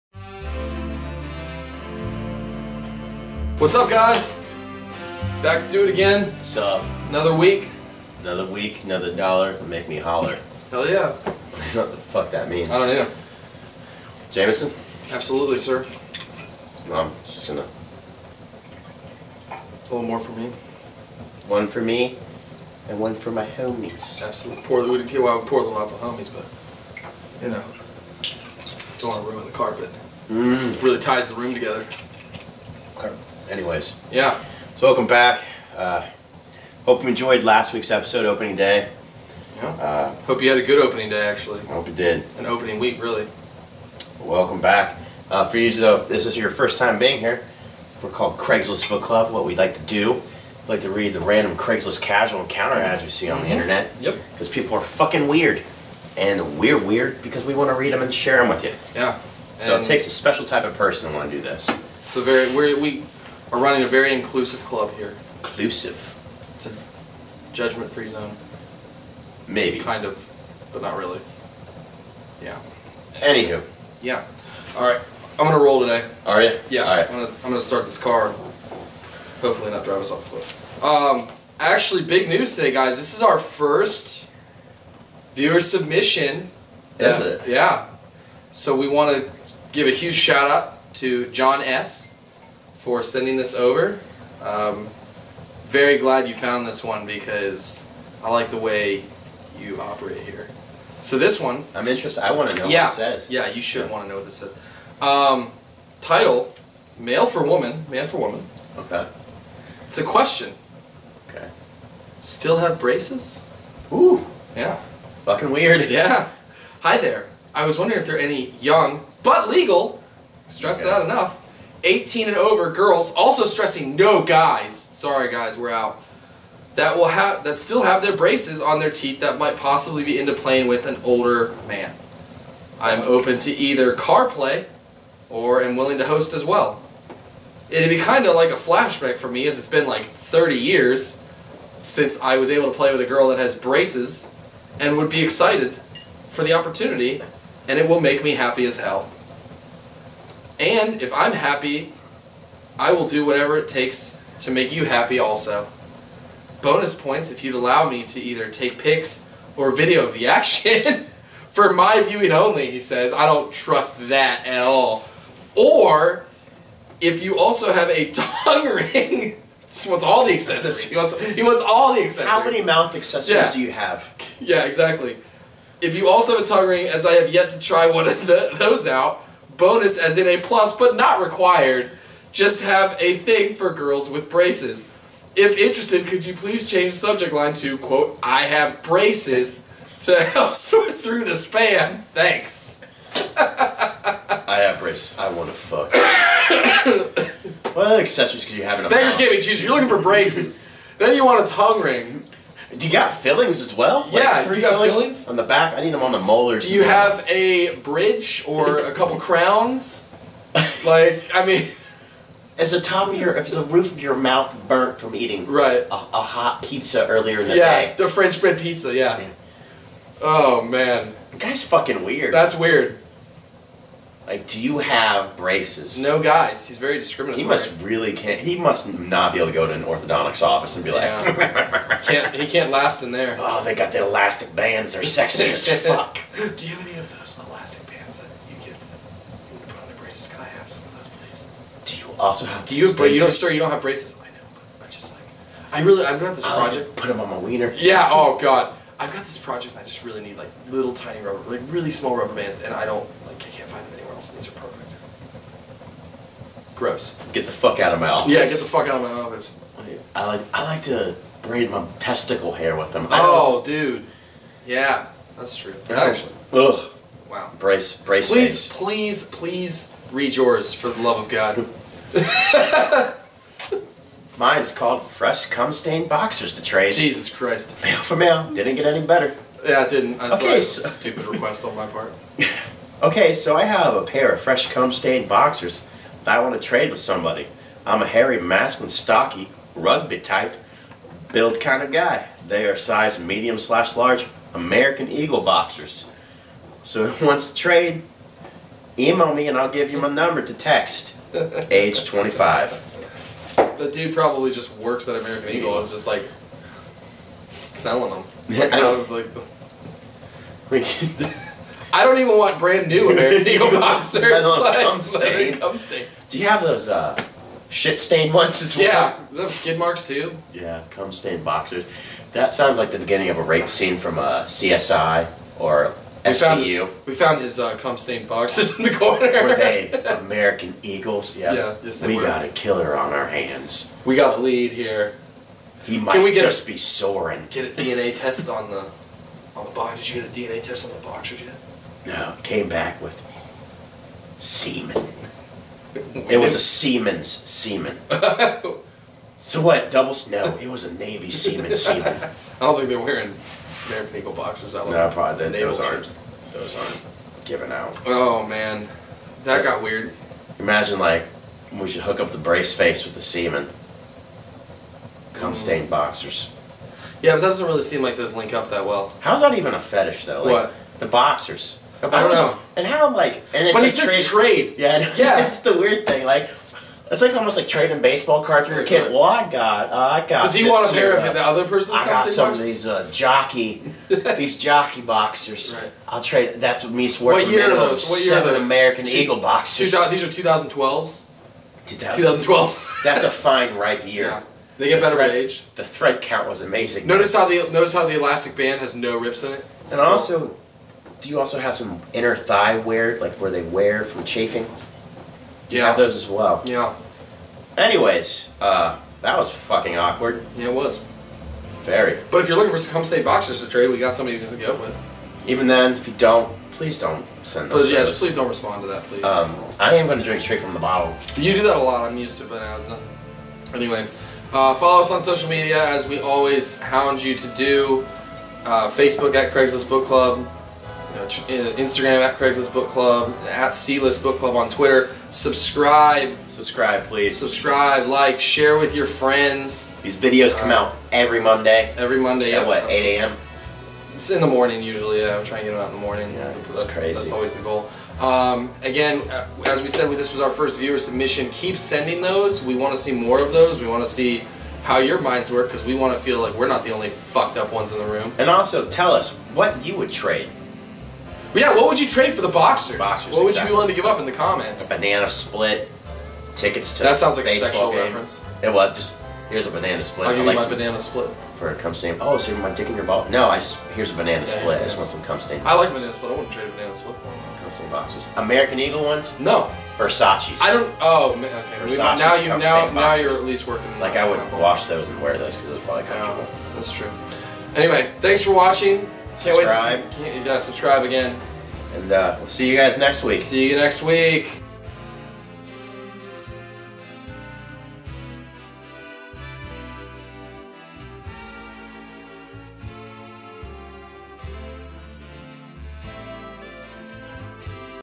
Girls With Braces & Boys Trading Boxers with Craigslist Book Club
In this week's episode the boys discover a guy who loves metalheads (literally) and a very naughty American Eagle Employee. Let us know what you would trade for the boxers below!